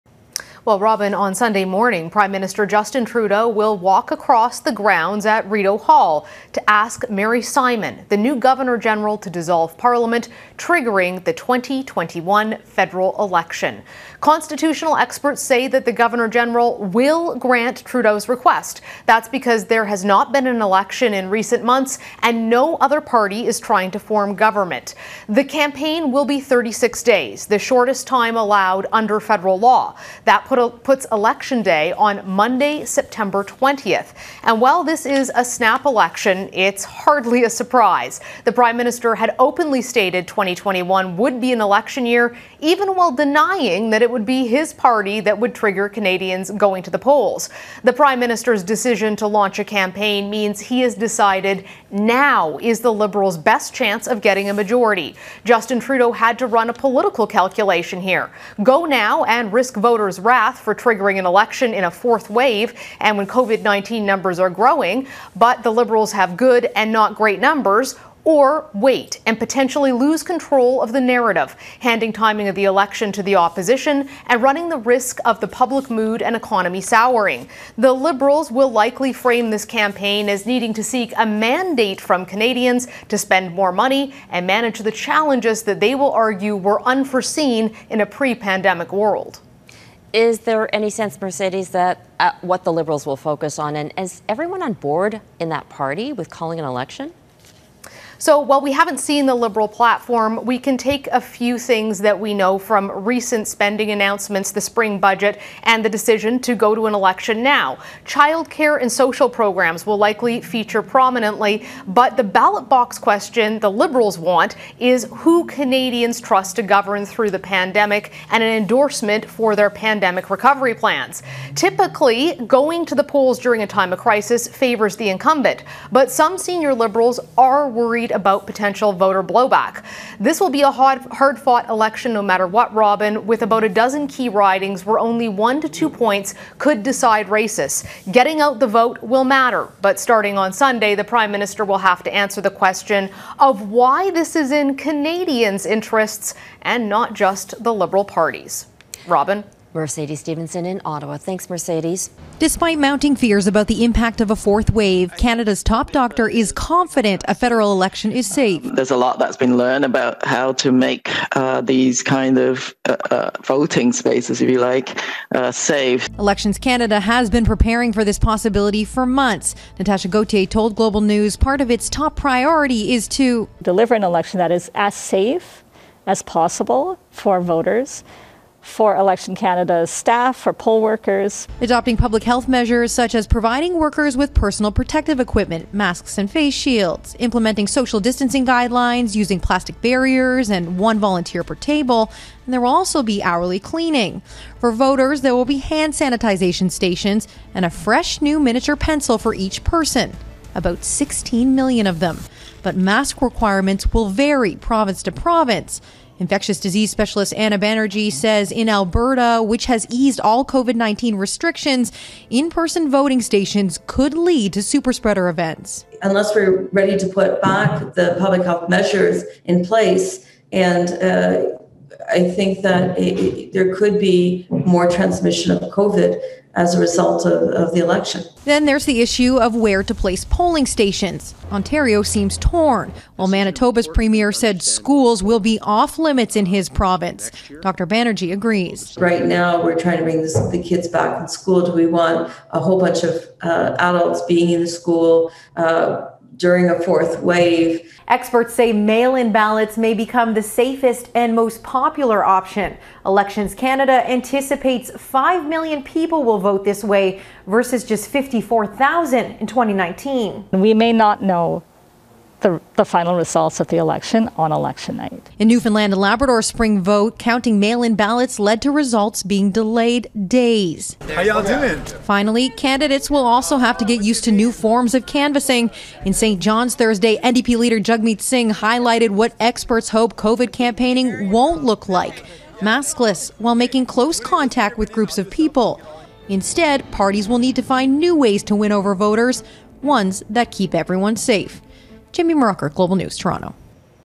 Concerns about holding federal election during 4th COVID-19 wave
With a federal election expected to be called for Sept. 20, there's growing concern over whether running a campaign during a fourth COVID-19 pandemic wave is safe.

Mercedes Stephenson explains why now may be the best time for the minority Liberal government to drop the writ.

Meanwhile, Jamie Mauracher takes a look at how the COVID-19 pandemic could change the process of how people vote.